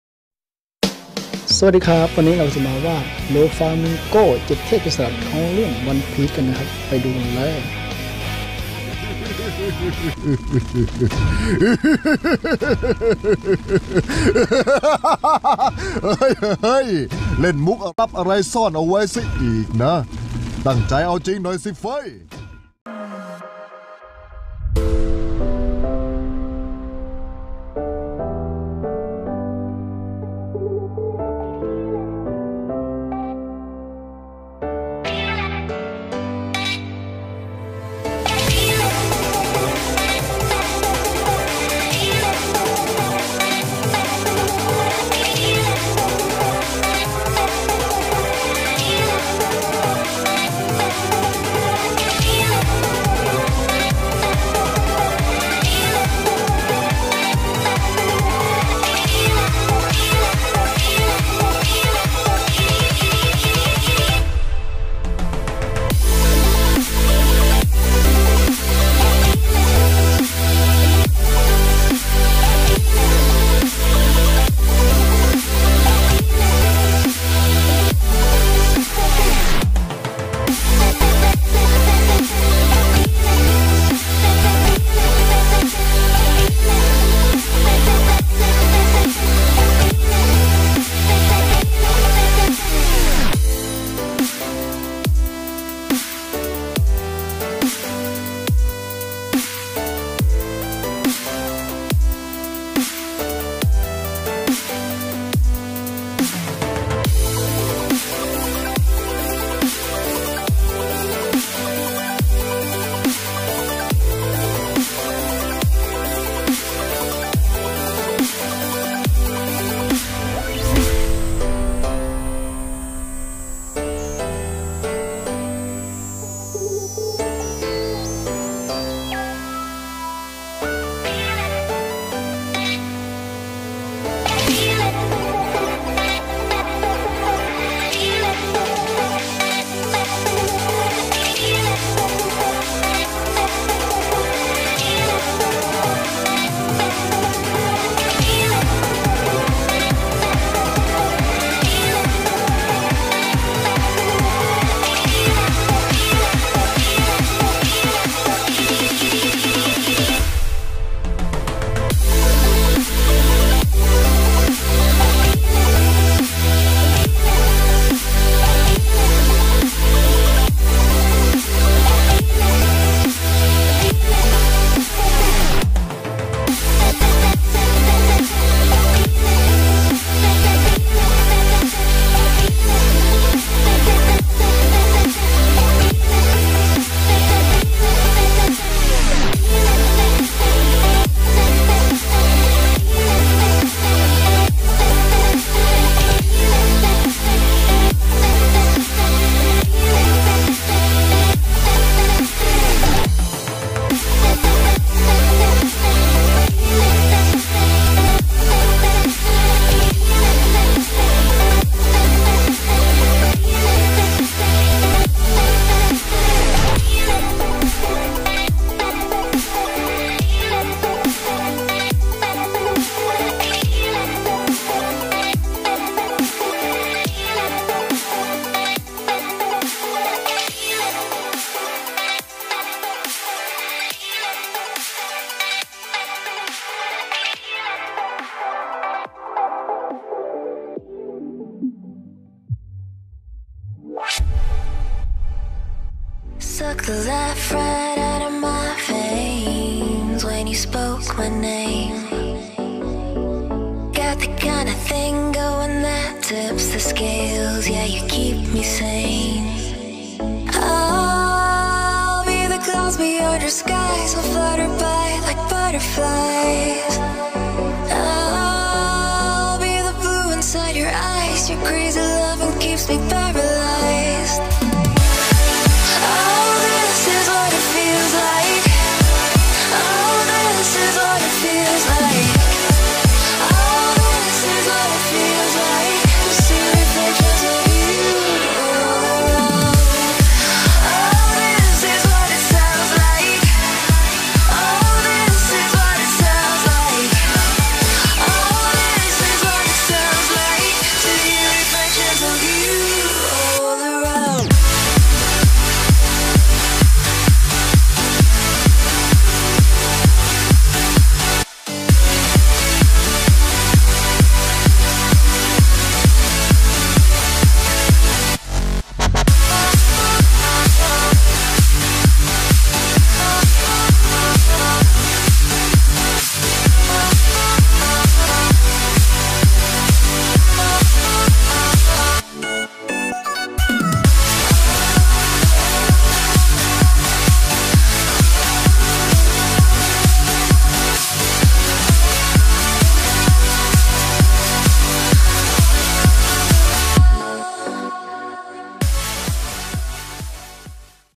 วาดรูปโด ฟลามิงโก้ เจ็ดเทพโจรสลัด จากเรื่องOne Piece
เขาคือ7เทพโจรสลัดหรือโจ๊คเกอร์